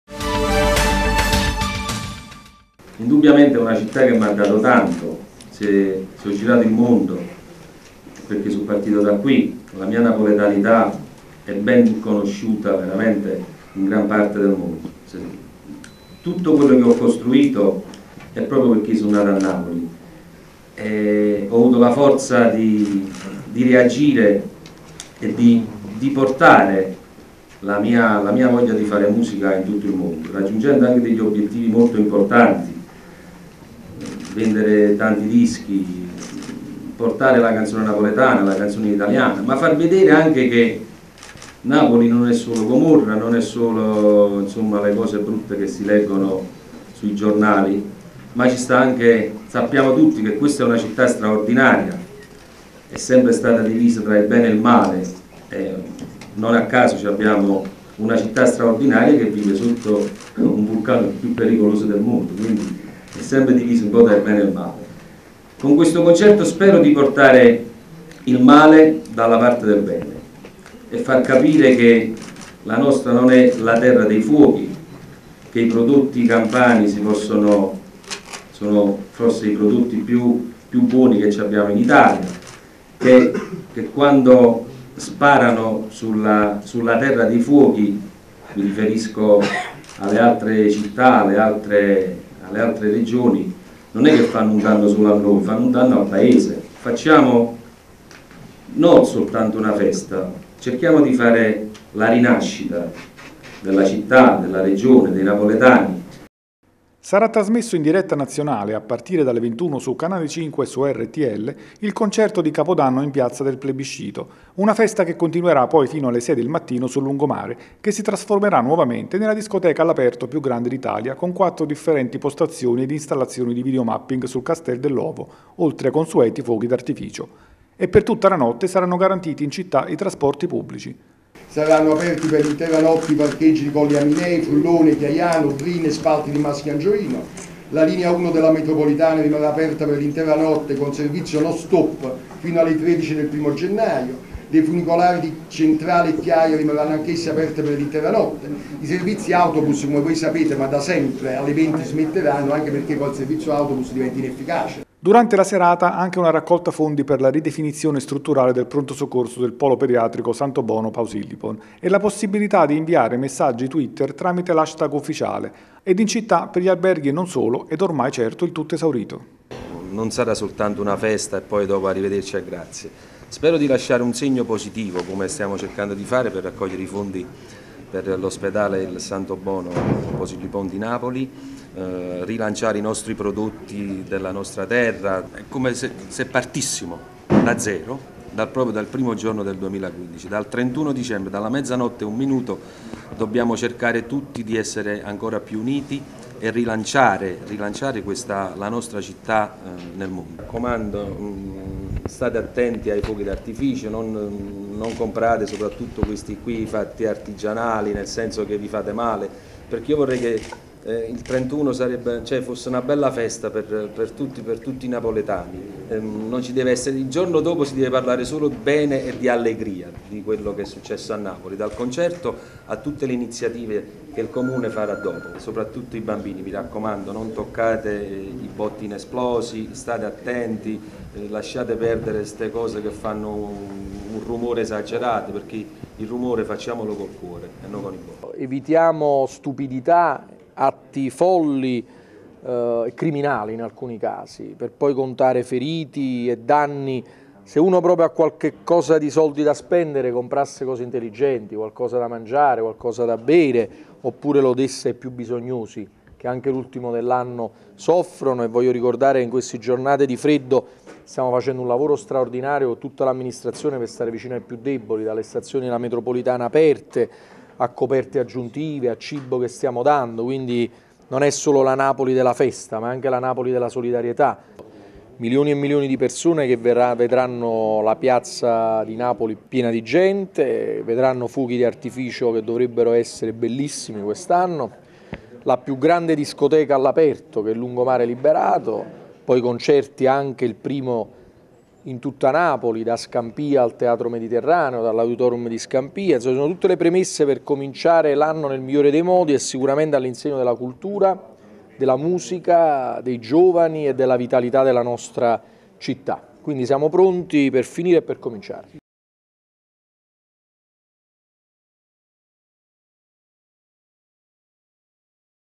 Capodanno 2015: tutti in piazza del Plebiscito e sul lungomare
Prima il concerto in Piazza del Plebiscito con Gigi D'Alessio & Friends trasmesso in diretta nazionale, poi fino al mattino sul lungomare, con fuochi d'artificio, video mapping sul Castel dell'Ovo e 4 postazioni musicali.
Ed inoltre, trasporti pubblici aperti tutta la notte, raccolta fondi per il Santobono Pausilipon, diretta twitter e tanto altro, con la città che registra già il tutto esaurito.